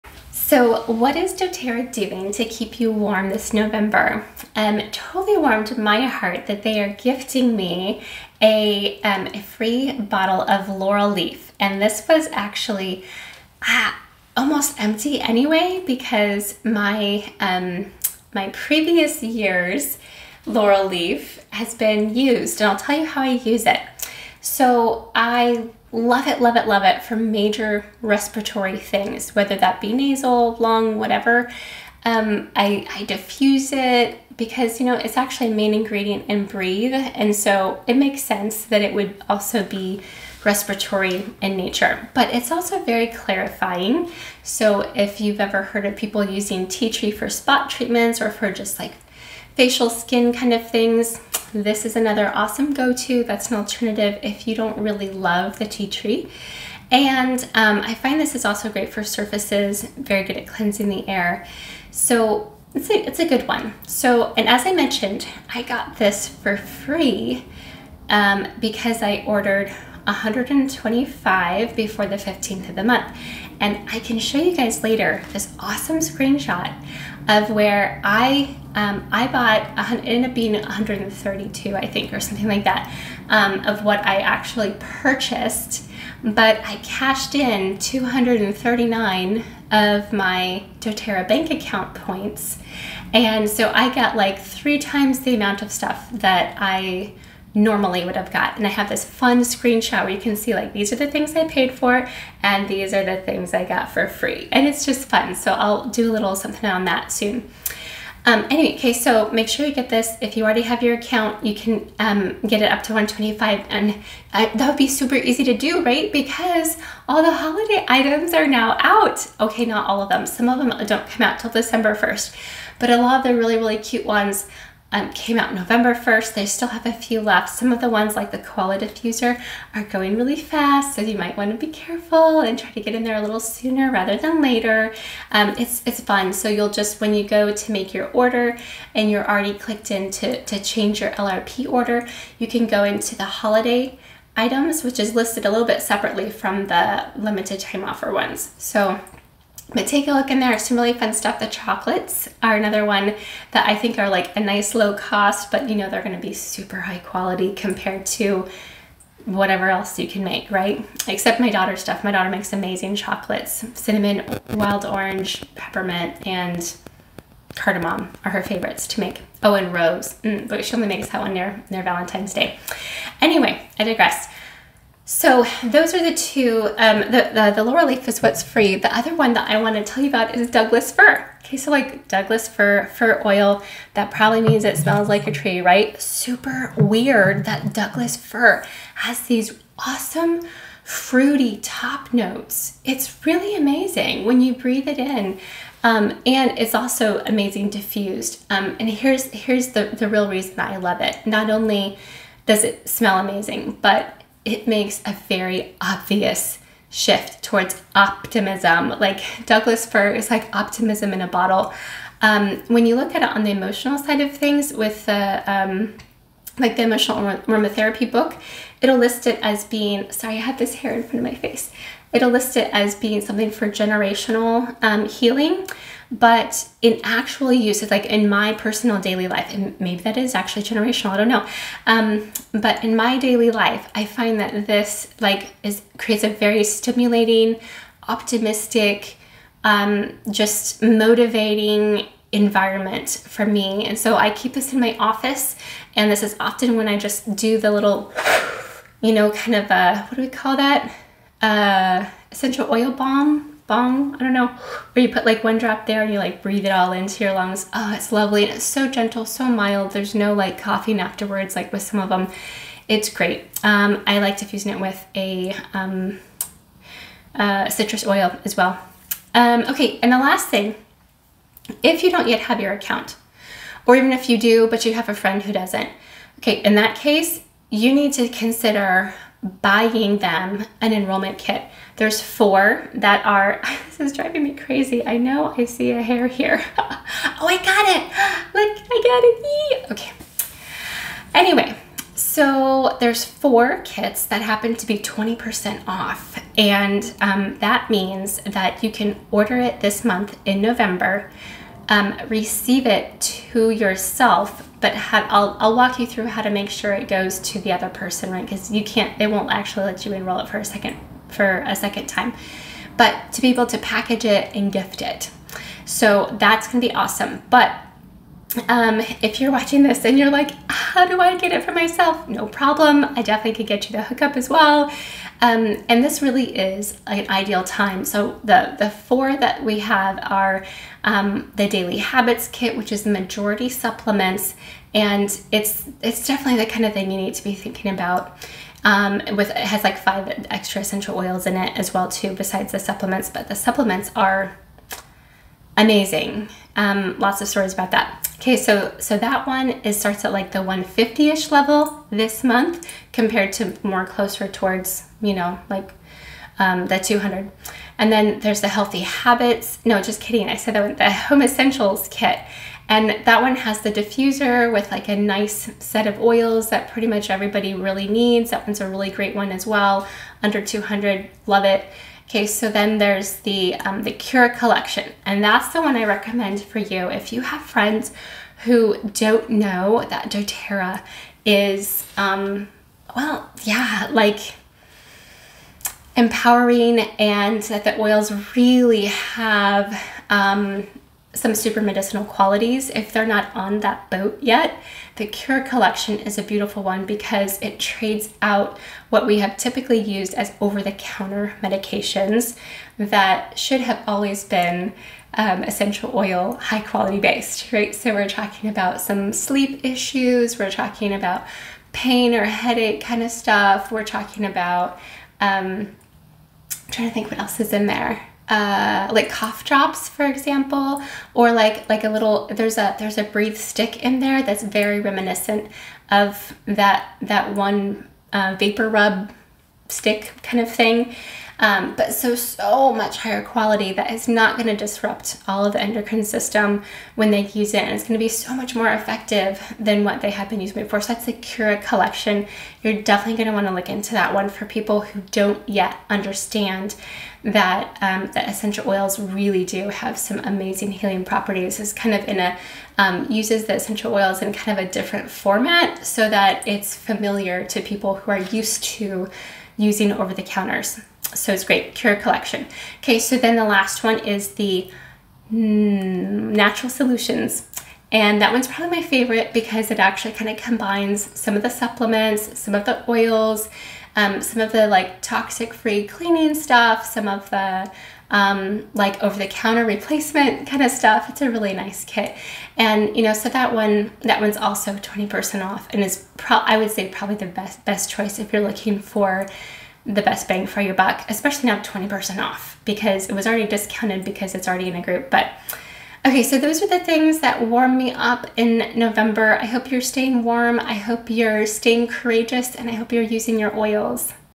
😍Warm Feelings for doTERRA in November 2021
What are the specials? Why is this a great month to get doTERRA products into your home? Connect with Amanda to get more info or try samples. fill out this form for samples